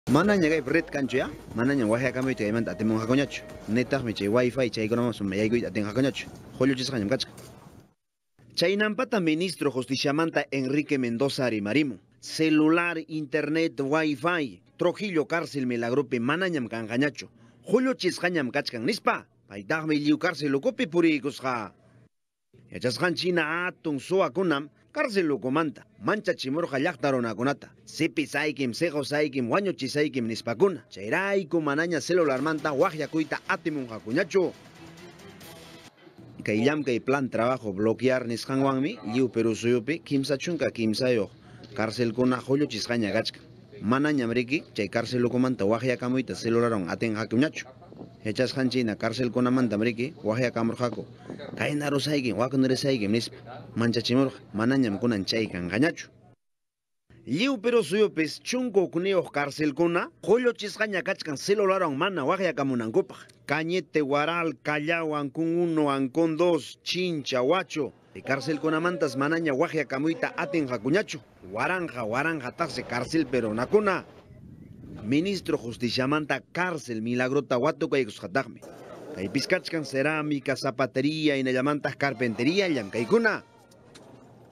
Trujillo: Carcel El Milagro ukumanta manaña suwakuna teléfono celularwan ...
Trujillo: Carcel El Milagro ukumanta manaña suwakuna teléfono celularwan waqyakuyta atinmunqakuñachu. Astawan yachaykunaykipaq qawaykuy.